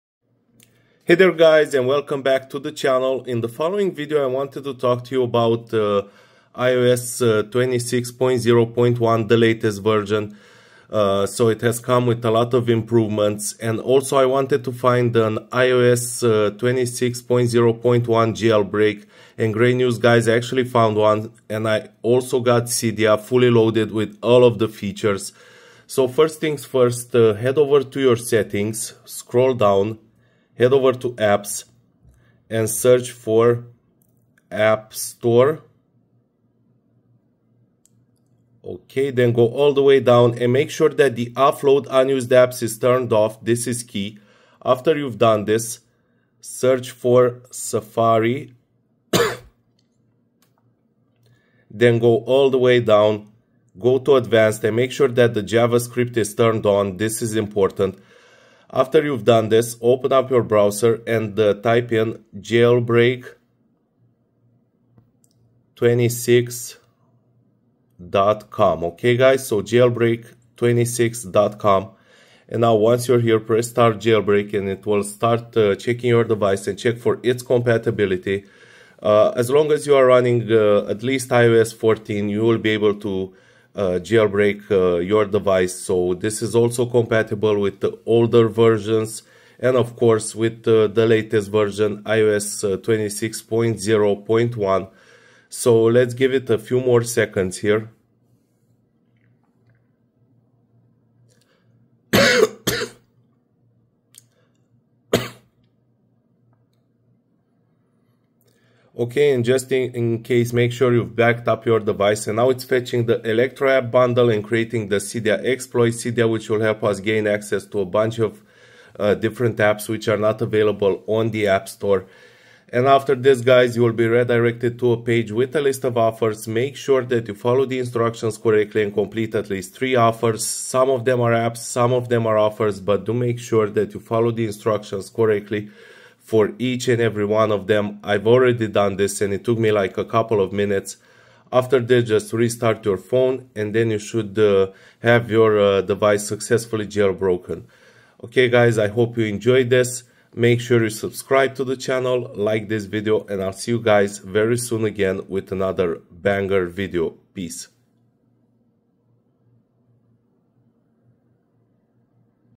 iOS 26.0.1 Jailbreak - How to Jailbreak iOS 26.0.1 Untethered Cydia
Hello everyone, today I'll be talking to you about how to jailbreak ios 26.0.1 completely untethered. There's been tons of requests that we do a video on jailbreak ios 26.0.1. There's been quite a few improvements on this version and we can highly recommend it. iOS 26.0.1 has just been released and it's better than ever before. Taking everything into consideration, make sure you've backed up your device for ios 26.0.1 jailbreak no computer to take full effect.